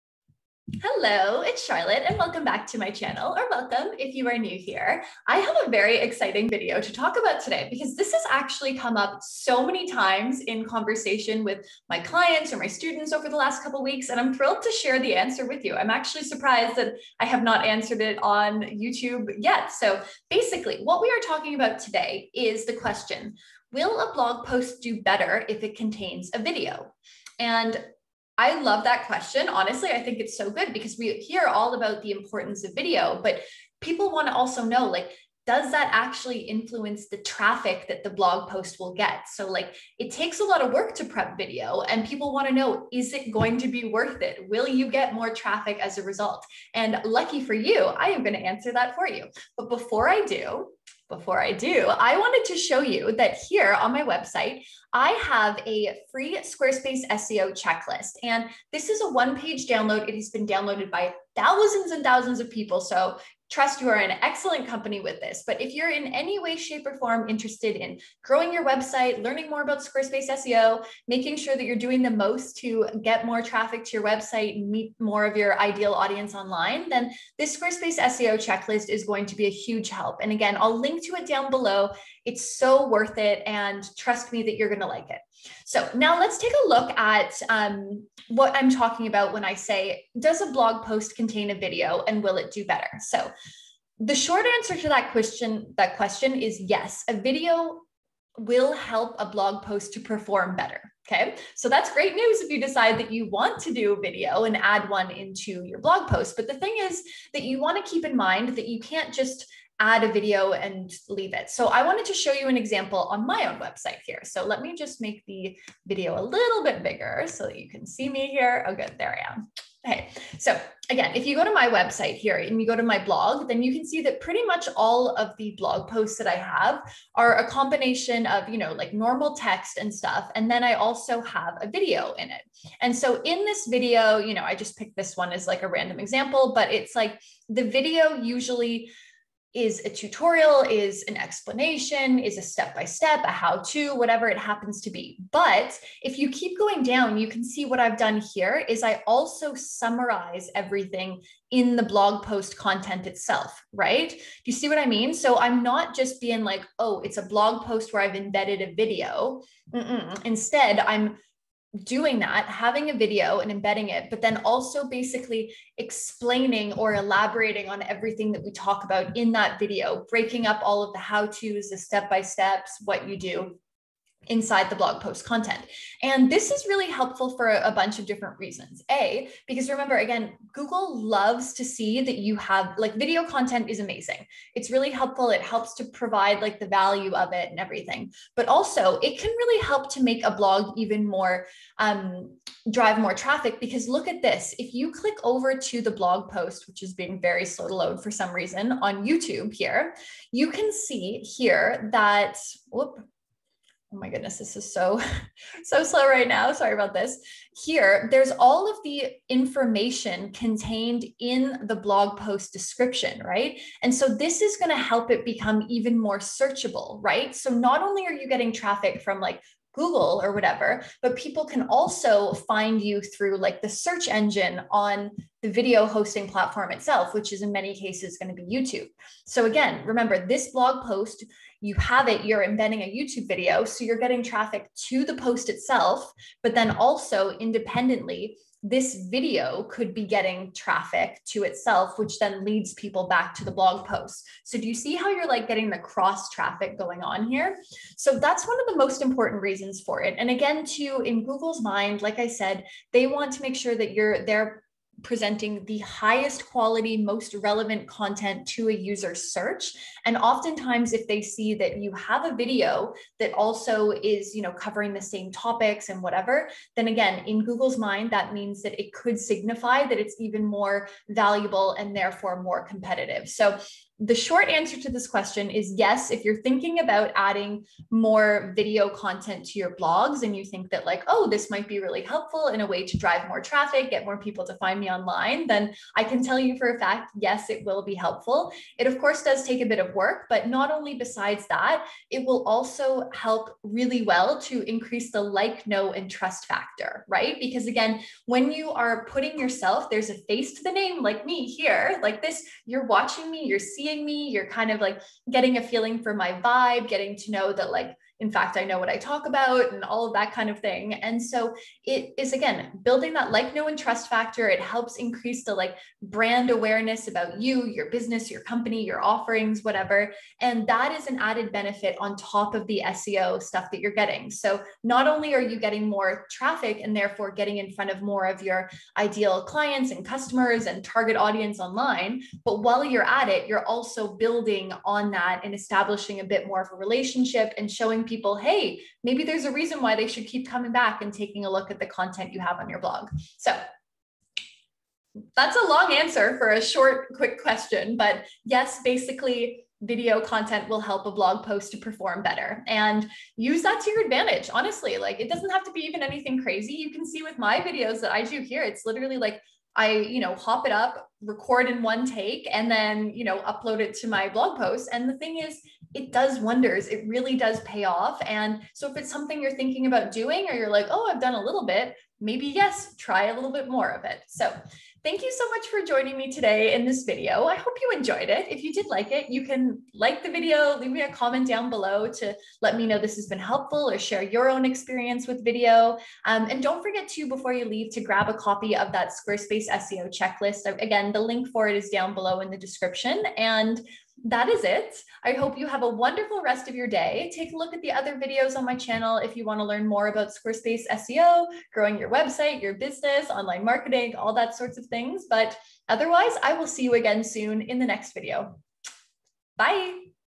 Do blog posts with videos get higher website traffic? | SQUARESPACE SEO
Do blog posts that contain a video do better & get more traffic?

This is a FANTASTIC questions and frankly, I'm surprised it's taken me this long to answer!

If you're spending any amount of time & energy blogging, you want to make sure that you're getting the most out of it, right? And you might have heard that one of the best ways to get a blog post to perform better (i.e., get more traffic and show up higher in Google search results) is if that blog post contains a video.

But is that really true? If so, will any old video do? I'm here to answer that question today!

Time stamps:
0:00 Intro
0:51 Squarespace SEO Checklist
1:26 The answer: Blog posts & video
1:53 Example on my website
3:08 More clicks from Youtube
4:12 Google likes video
4:50 Video helps with Like, Know & Trust
5:43 Outro

-Charlotte